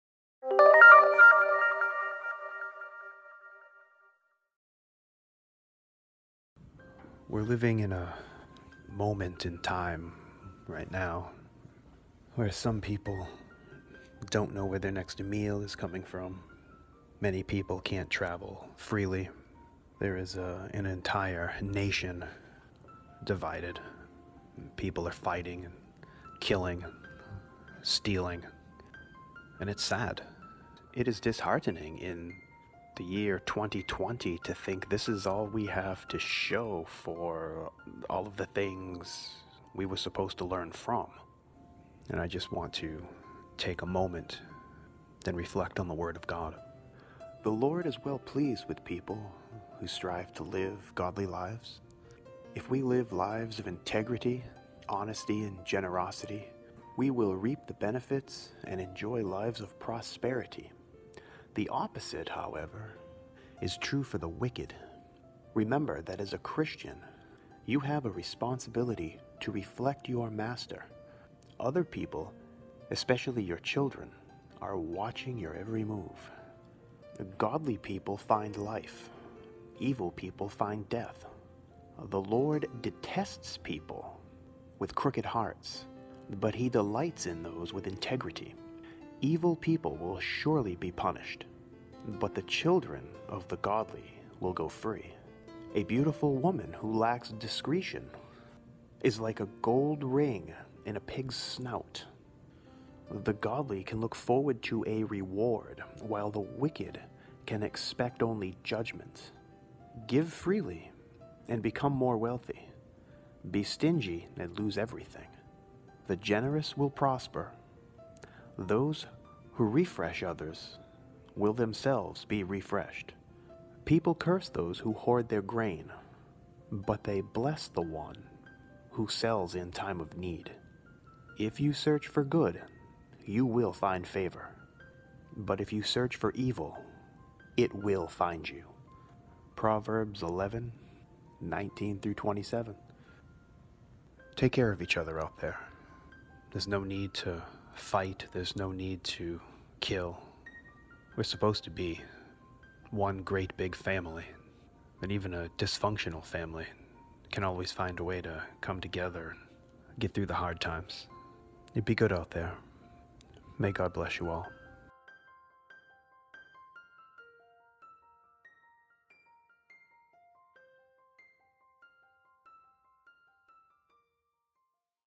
Peace Through Integrity for the Greater Good of God
I know there is a lot of pain in the world right now and people are afraid, angry and sad.  Violence is no easy answer.  The message has been made, loud and clear.  Now is the time to let kindness towards each other prevail.  I pray for peace through Jesus Christ.  May God bless you all.